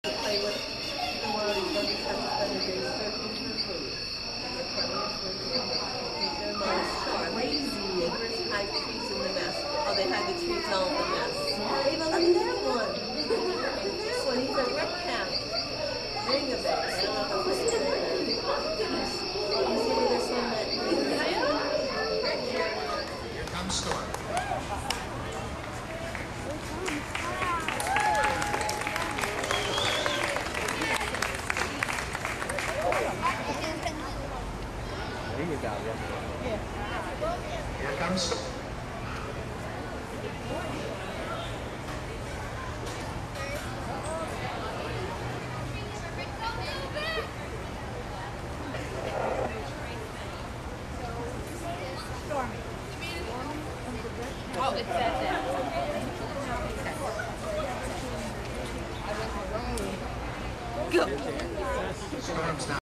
Philadelphia Zoo Treetop Trail Unveiling Ceremony (featuring Storm, the Redcapped Mangabey)
Before the ceremony, Storm prepared by eating breakfast in his indoor exhibit.  Then, right on cue, he entered Treetop Trail and walked through the mesh tunnel to show the Zoo's Board of Directors, the donors, and special invited guests,  how this new exhibit feature works.